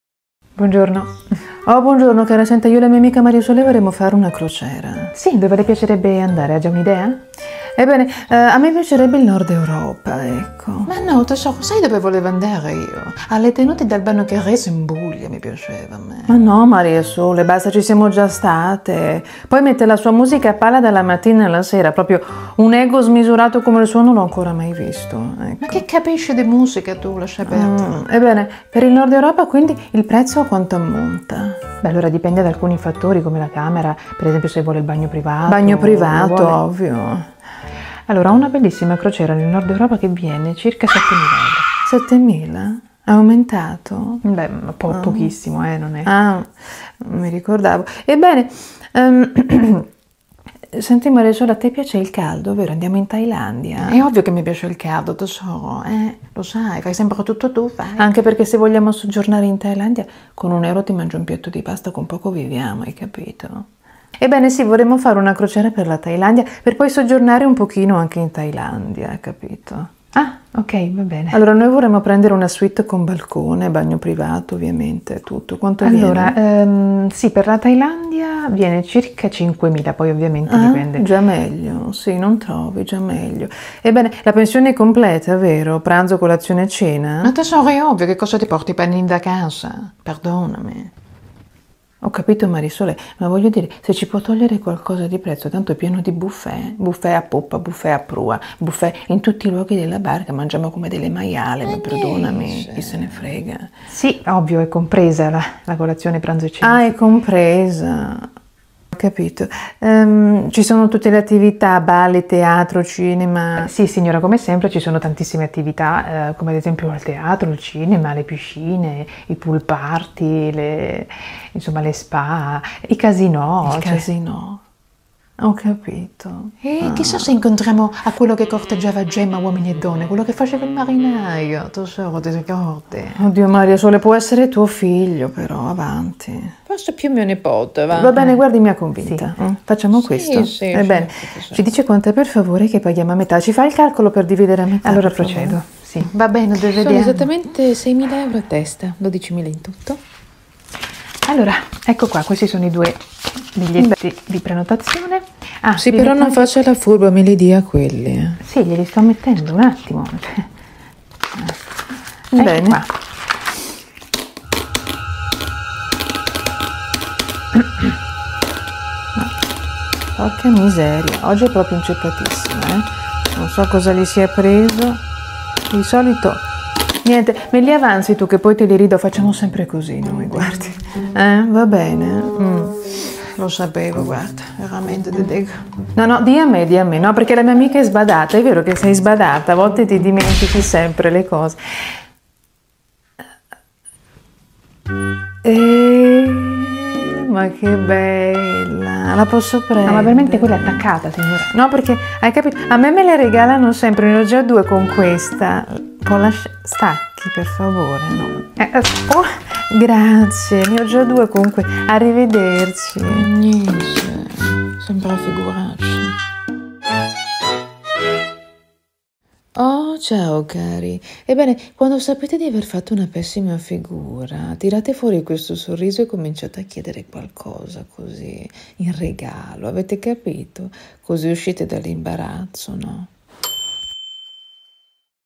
La miliardaria in agenzia viaggi
Oh cari…. Ho voglia di farmi  una bella crociera…. 🛳️ non vedo l’ora di salire sulla nave e mangiare a sbaffo 😎🫶🏼